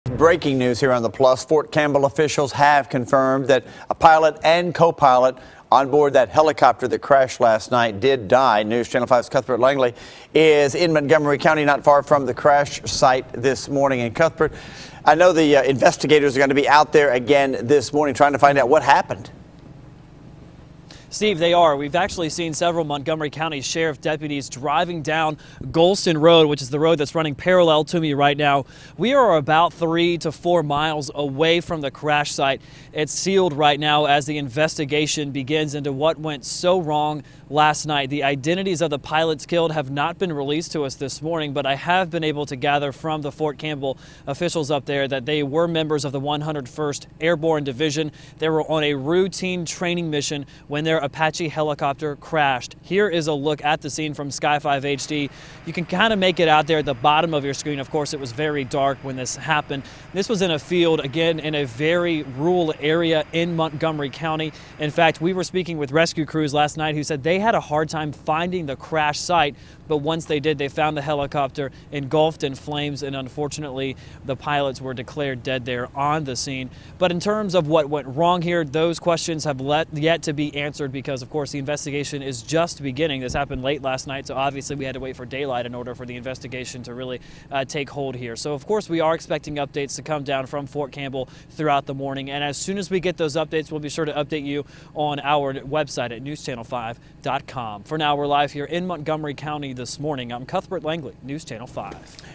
Two Fort Campbell Pilots Killed in Apache Helicopter Crash in Clarksville
Two Fort Campbell Pilots were killed in a helicopter crash during a training mission last night in Clarksville.